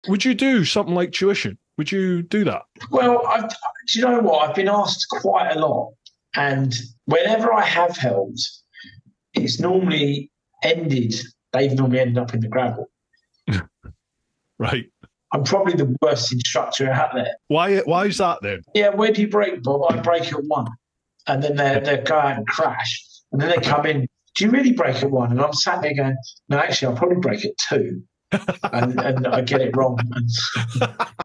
WHERE do you Brake Bob?
Bob Collins is a British motorcycle racer and owner of Motorcycle Service centre and tyre specialists FWR Ltd. He's raced at almost every level within the UK motorcycle racing scene and beyond into Europe, with stories to match!